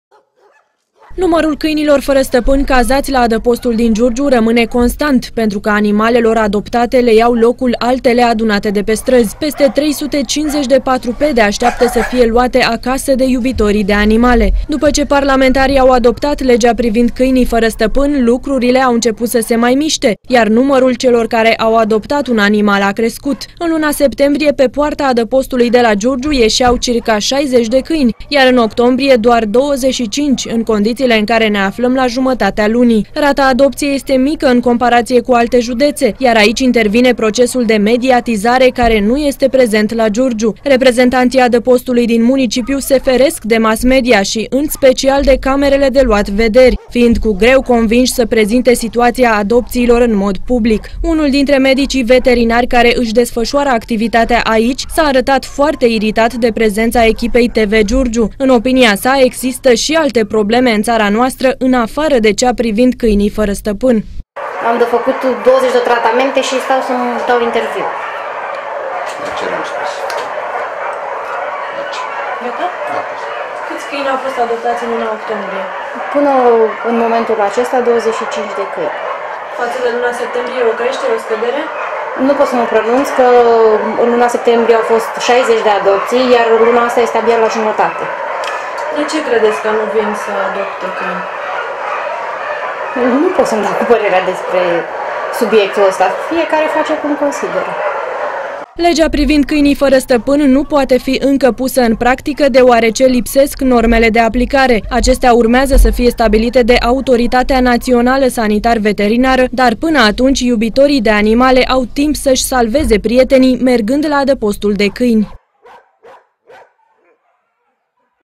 IN CONTINUARE FARA STAPANI
Dacă în alte județe din țară, adopția câinilor este mediatizată și încurajată de autorități și asociații de protecție a animalelor, la Giurgiu situația stă altfel. Aici, reprezentanții adăpostului canin nu depun niciun efort pentru a face cunoscut numărul câinilor care pot fi adoptați. Cu toate acestea, sunt și giurgiuveni care iau acasă un patruped.